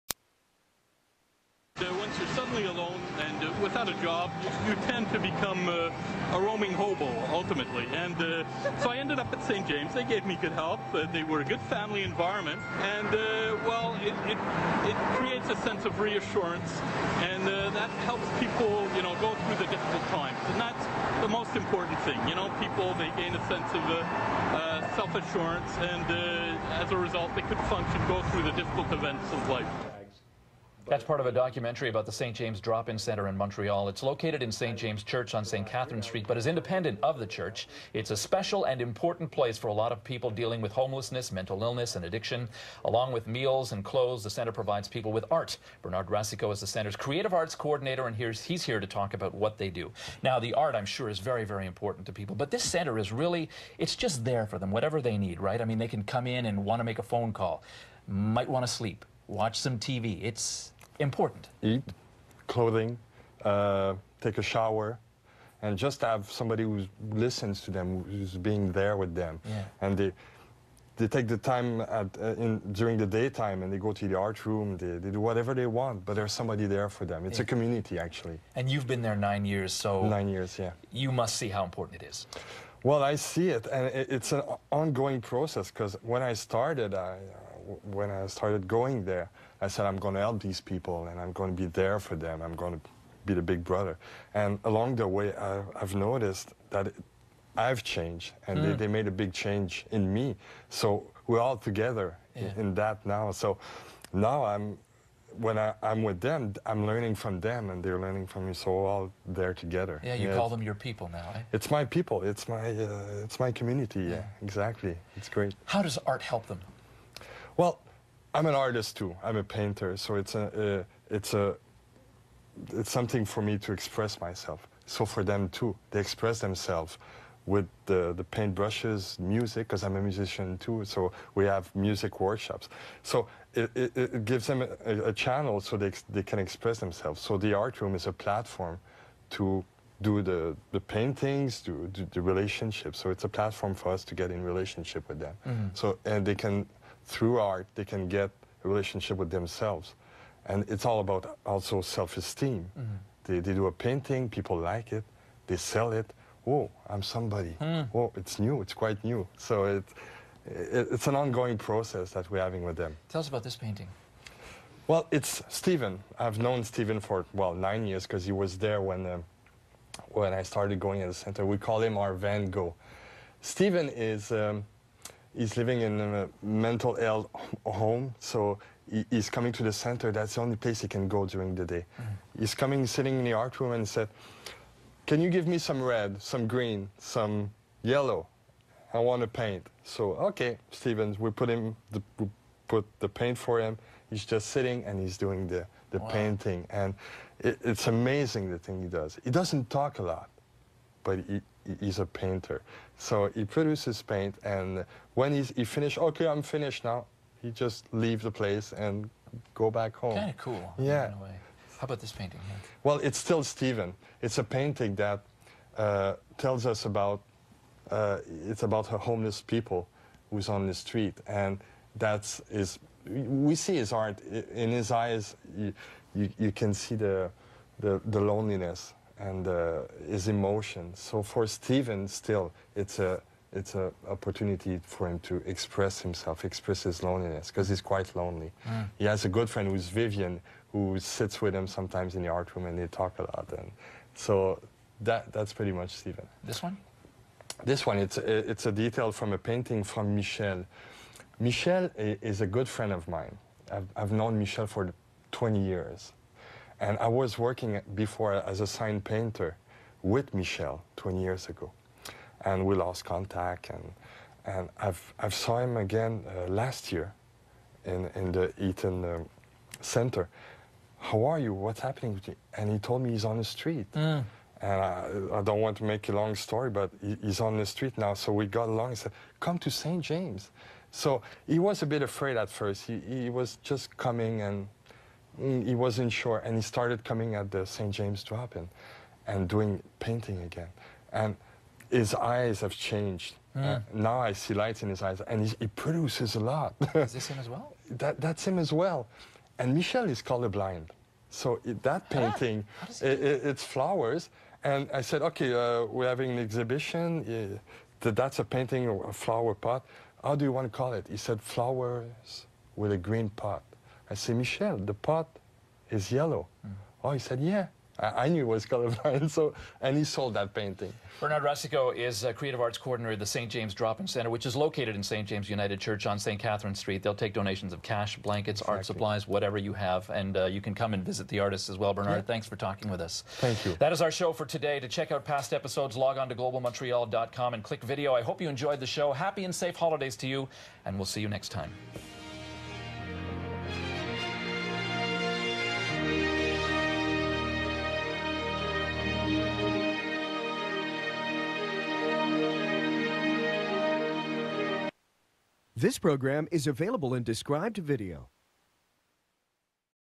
Montreal Homeless Doc GlobalTV Promo
I was contracted to shoot a TV documentary on Montreal's Homeless Men and it was promoted and shown on Global Television in Canada.